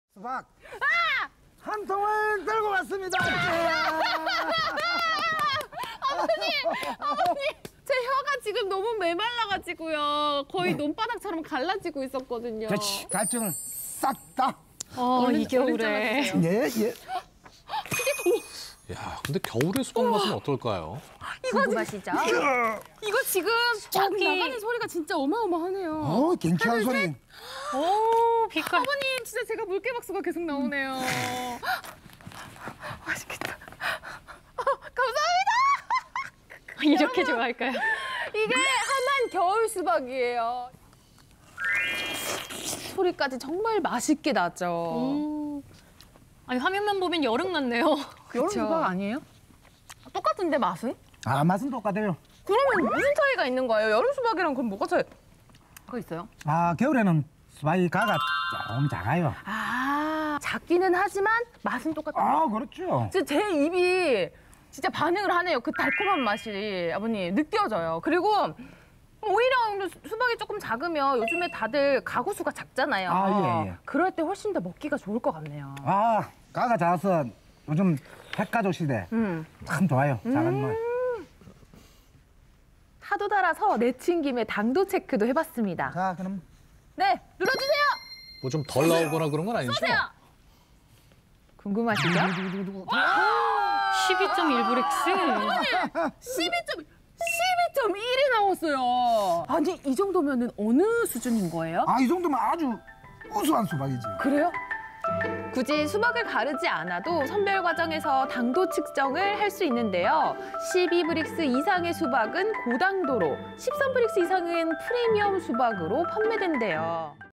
[숏클립] 겨울 수박의 본고장, 함안에서의 수박 먹방~ (2020.02.04, 화)
추워야 더 맛있는 철없는 겨울 수박을 만나러 장지현 리포터가 떴다!
함안에 겨울 수박이 가능한 이유는 완만한 평지와 풍부한 일조량, 적합한 토양이 있어서 가능합니다.

그렇다고 여름 수박보다 당도가 낮을 거라는 생각은 착각!
여름 수박과 겨울 수박의 차이점은 여름 수박보다 조금 작다는 것이 전부!
1인 가구 수가 늘어나는 요즘 겨울 수박이 더욱더 뜨고 있습니다~

또한 수박 껍질로 팩을 하면 수분 보충과 진정 효과가 있다고 합니다!
버릴 것 하나 없는 겨울 수박! 오늘 디저트는 겨울 수박이 어떨까요?